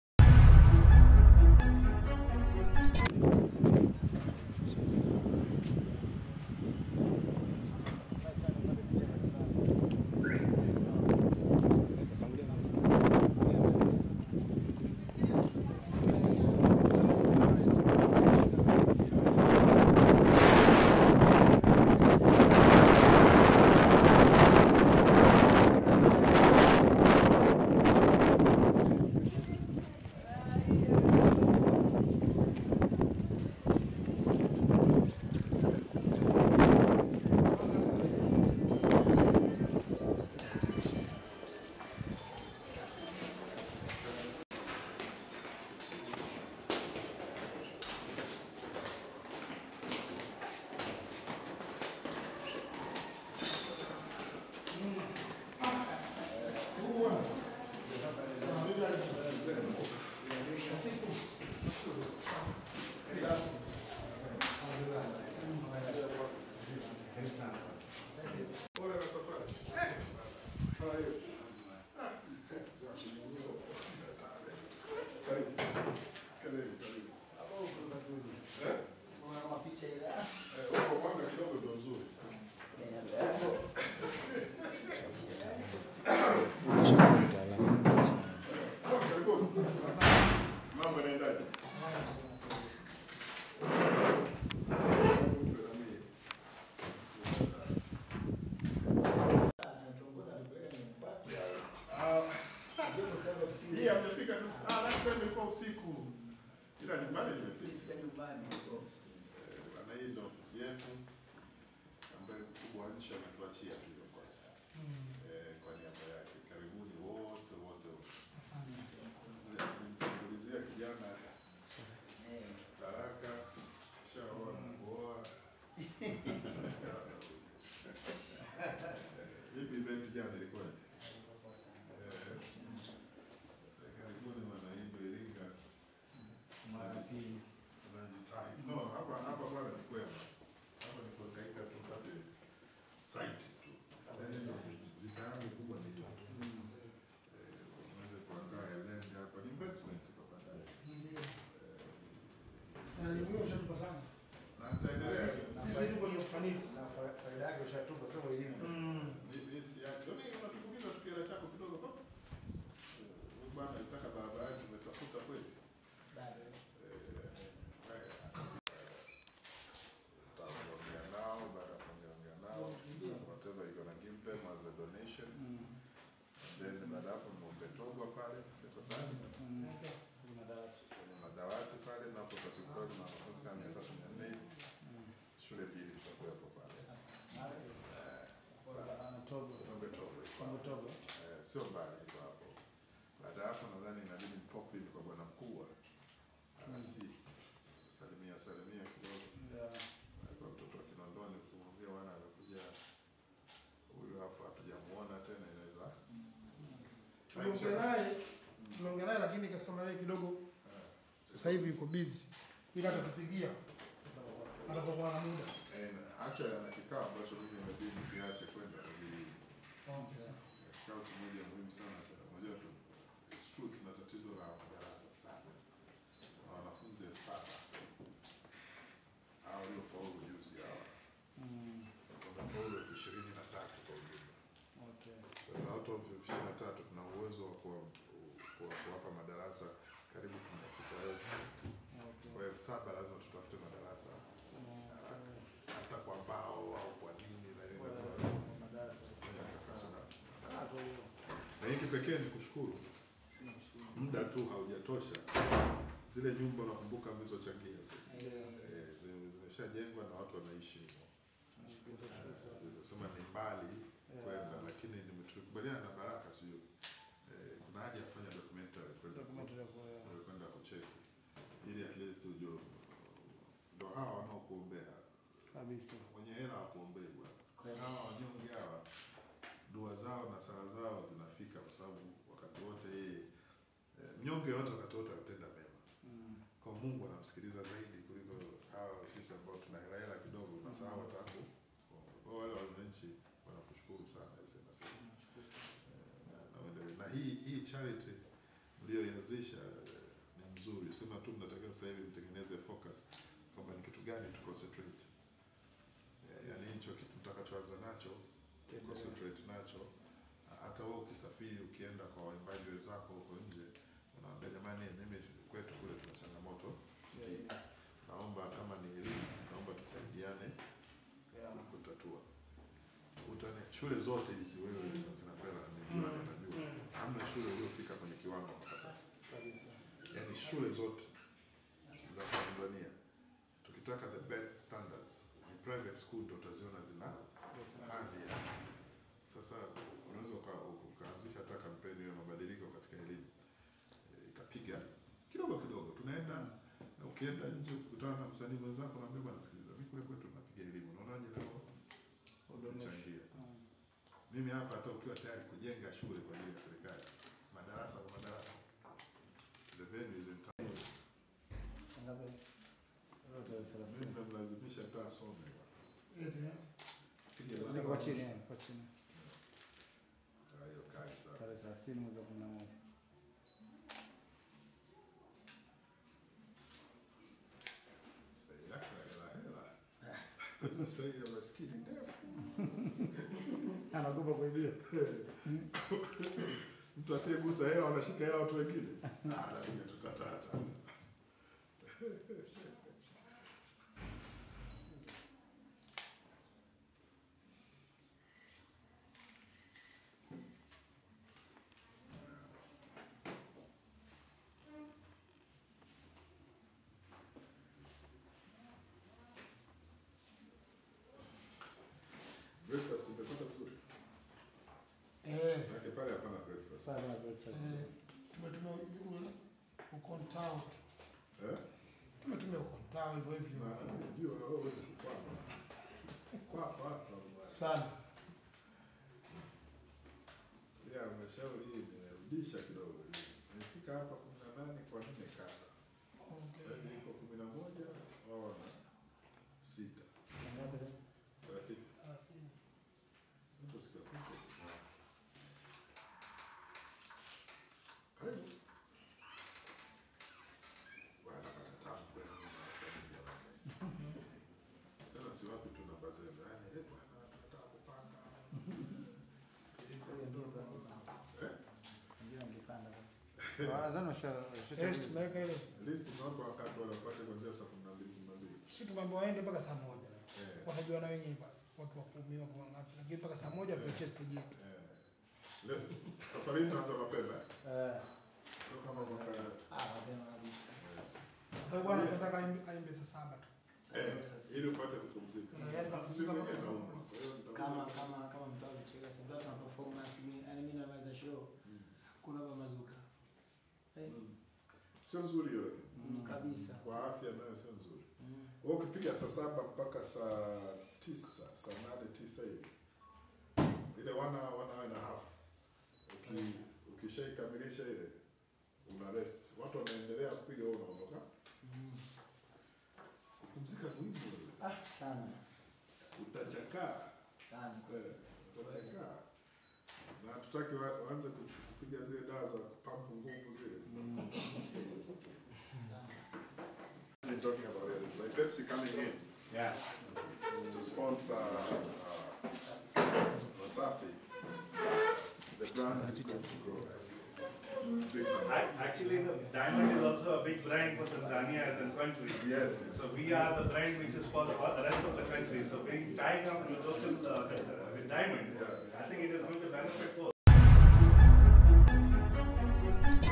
Diamond alivyotinga Ofisi za Mkuu wa Wilaya Iringa/Wasafi Festival Samora
Msanii wa muziki nchini DiamondPlatnumz ametembelea Ofisi za Mkuu wa Wilaya Iringa na kuzungumza na Mkuu huyo, Richard Kasesera katika kuelekea tamasha la Wasafi Festival uwanja wa CCM Samora